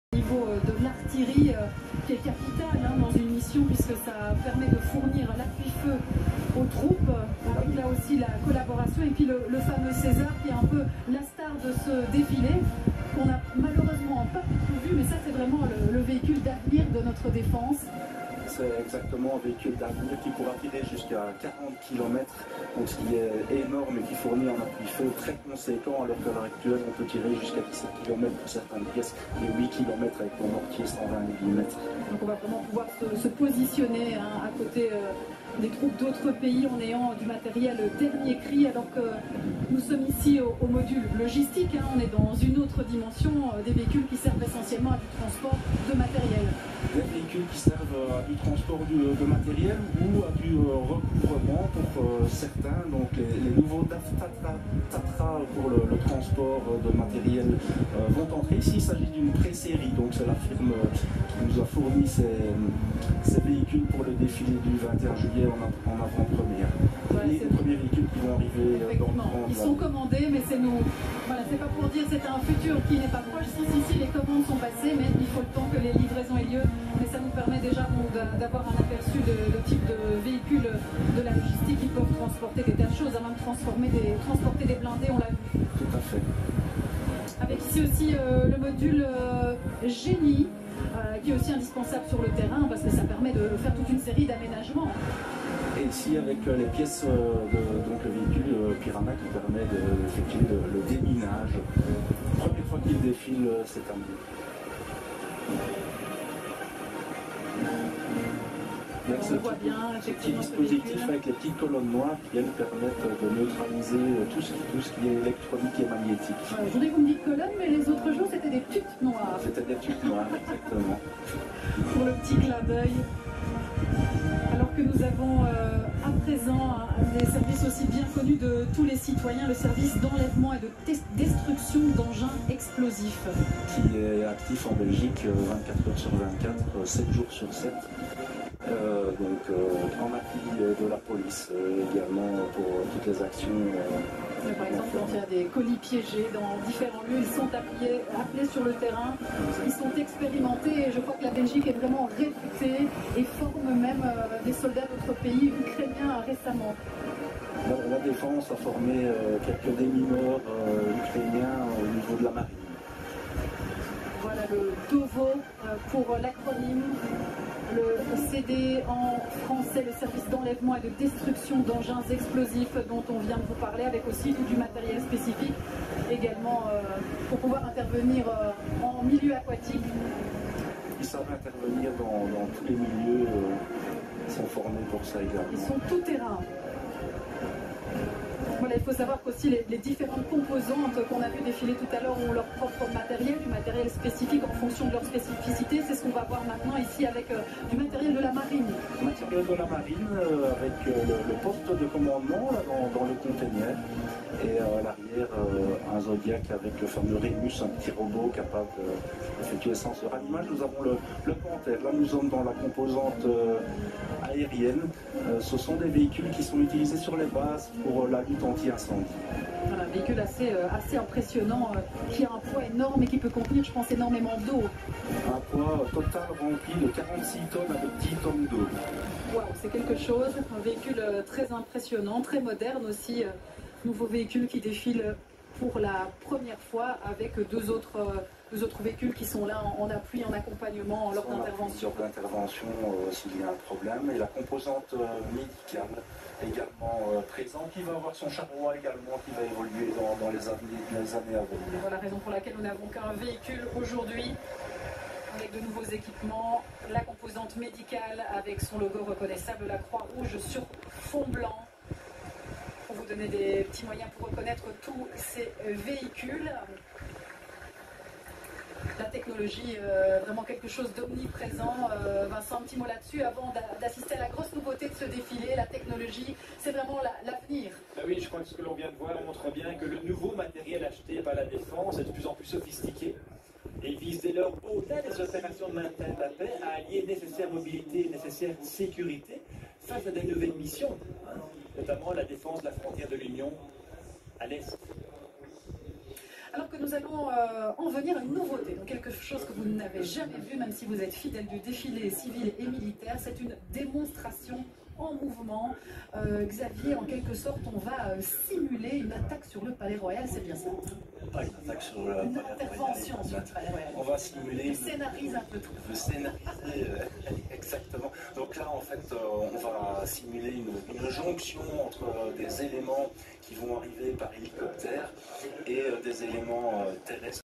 Cérémonie de la féte nationale. Belgique วันชาติเบลเยียม 🇧🇪 21/07/2022
โชว์รถ อุปกรณ์ทางทหาร